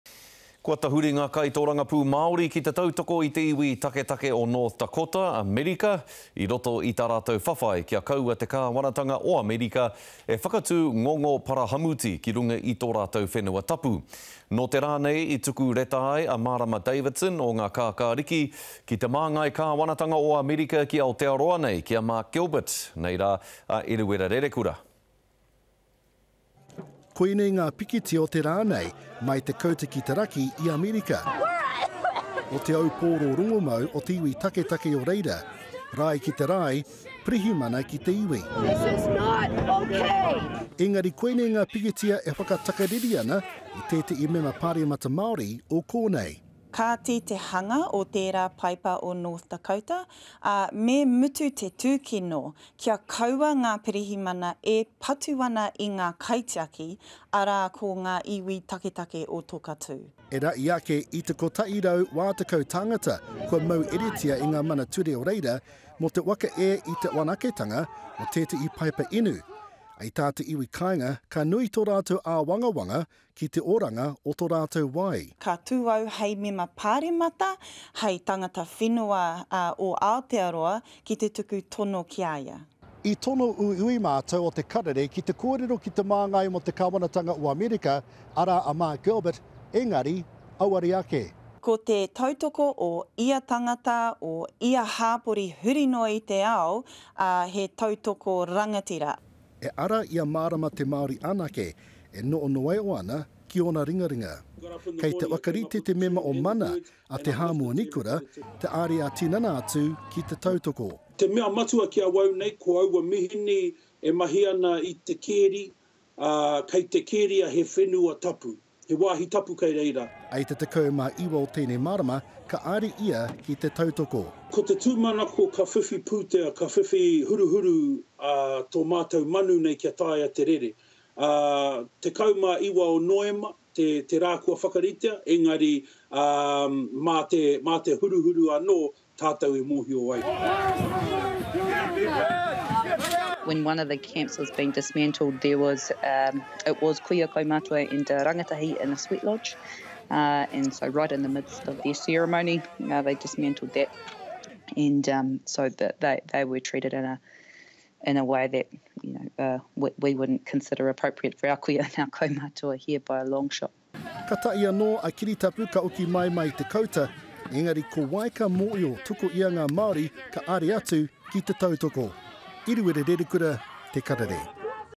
Māori support of Standing Rock steps up
Māori politicians have now joined the growing chorus of support for the First Nation's campaign to block a pipeline development over their sacred lands in North Dakota, America. Green MP Marama Davidson today sent an open letter to the United States Ambassador to New Zealand Mark Gilbert.